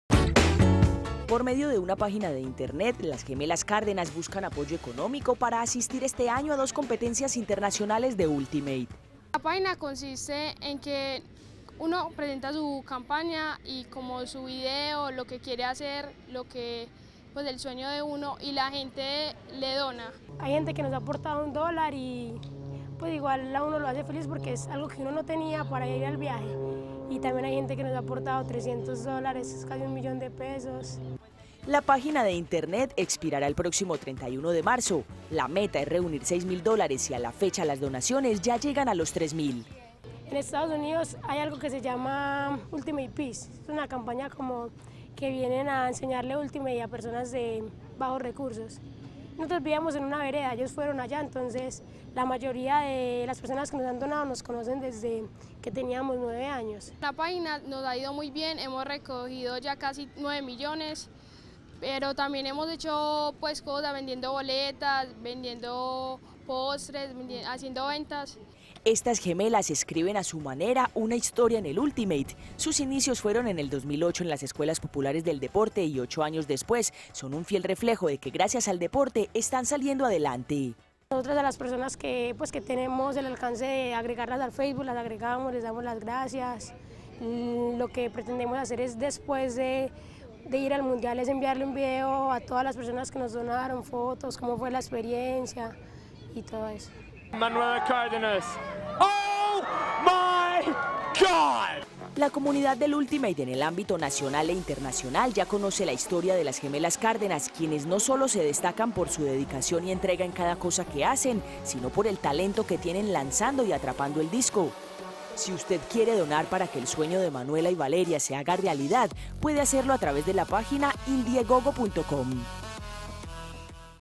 Las gemelas Cárdenas unidas por el ultimate, buscan apoyo económico [Noticias] - TeleMedellin
Estas gemelas escriben, a su manera, una historia en el ultimate, sus inicios fueron en  2008 en las escuelas populares del deporte y 8 años después son un fiel reflejo de que gracias al deporte están saliendo adelante.